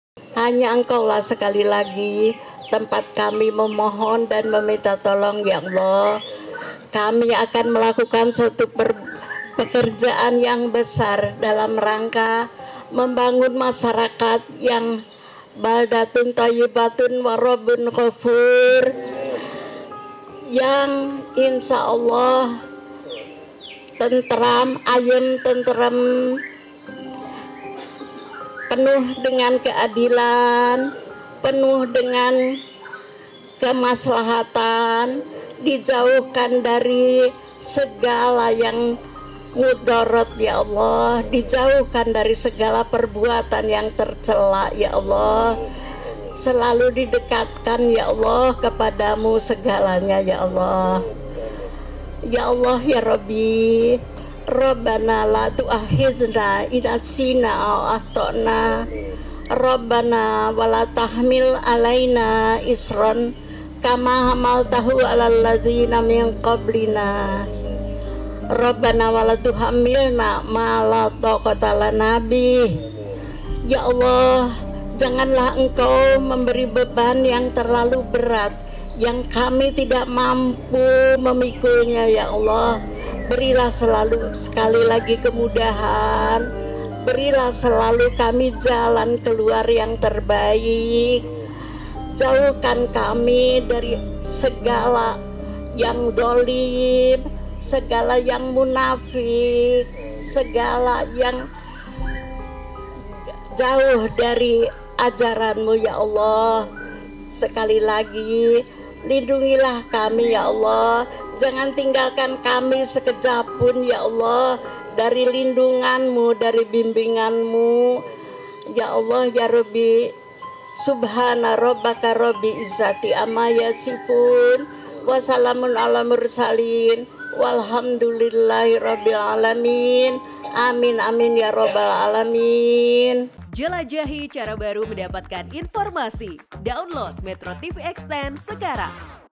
BREAKING NEWS - SEBELUM KE KPU, ANIES MUHAIMIN MINTA RESTU KELUARGA
Pasangan bakal calon presiden dan wakil presiden Anies Baswedan dan Muhaimin Iskandar berangkat ke KPU RI untuk mendaftar Pilpres 2024 pada Kamis (19/10/2023) pagi. Sebelum berangkat, sekitar pukul 05:22 WIB, Anies-Muhaimin melakukan prosesi meminta restu ibunda dan keluarga.